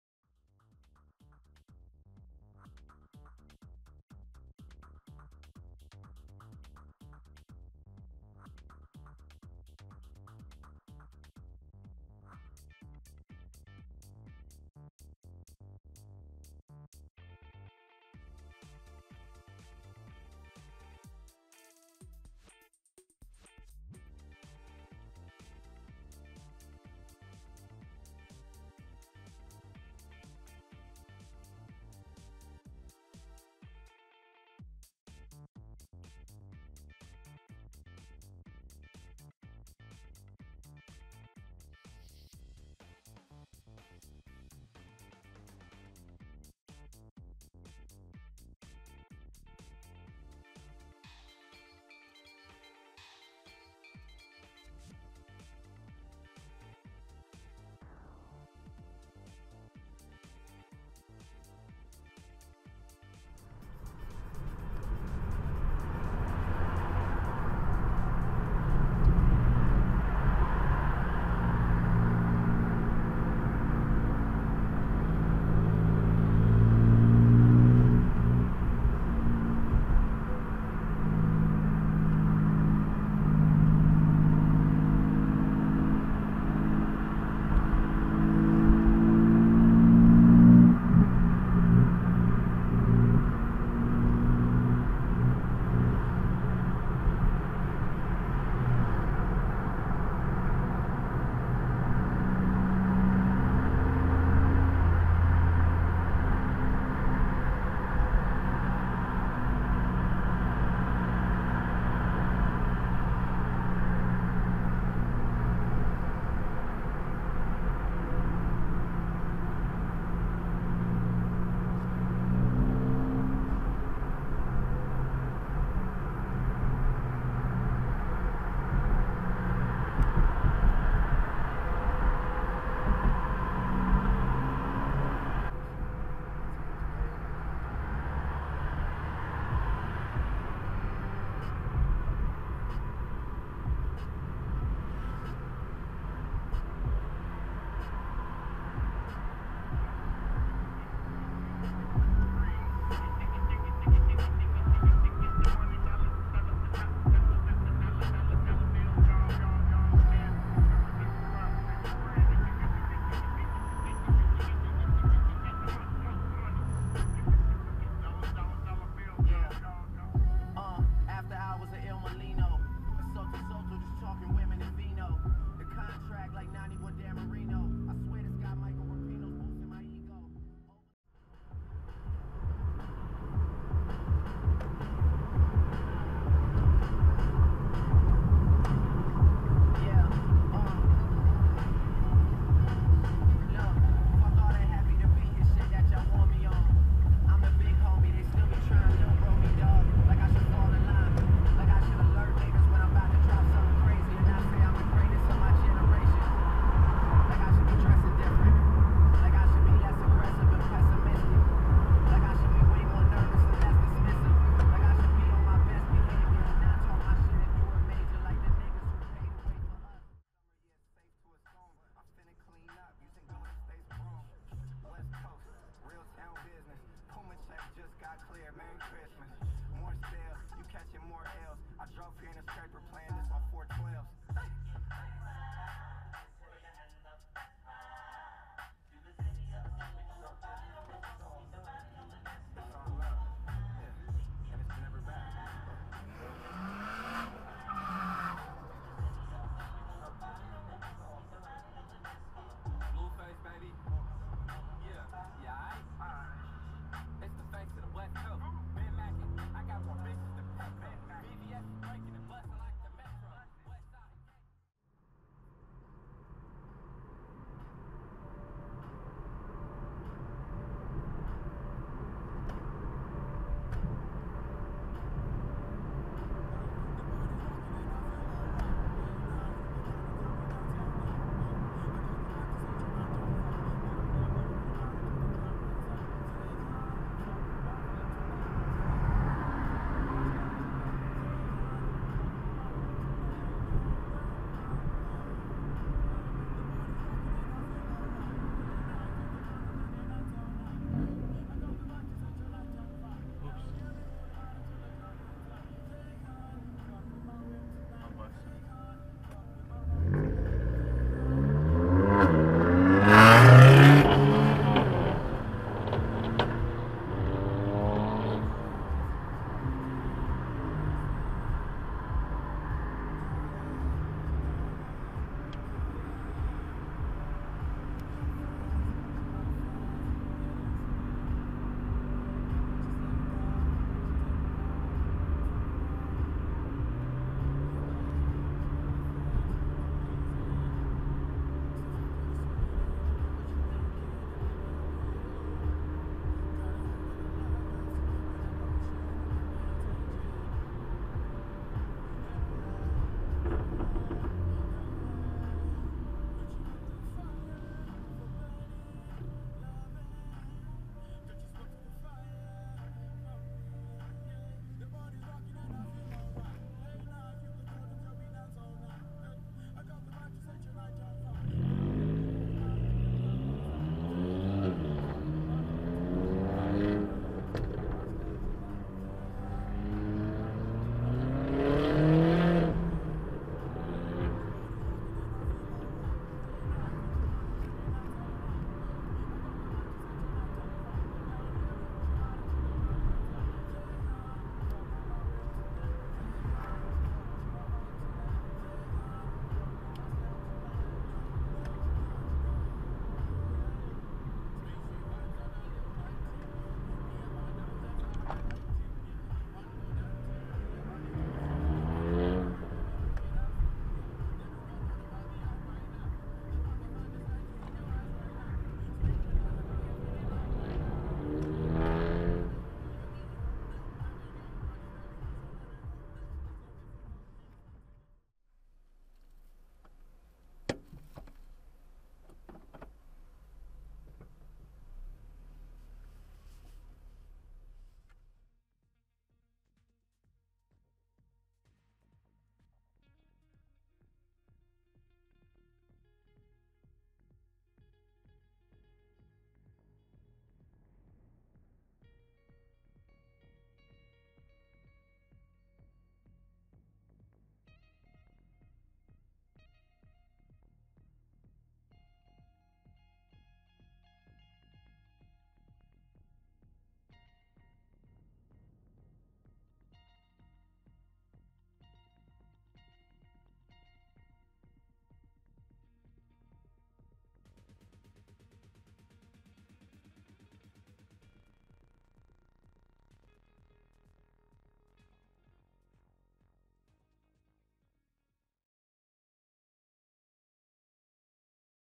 North Curl Curl | Midnight Run | RFR
Video of the RFR Midnight Run in May. Enjoy!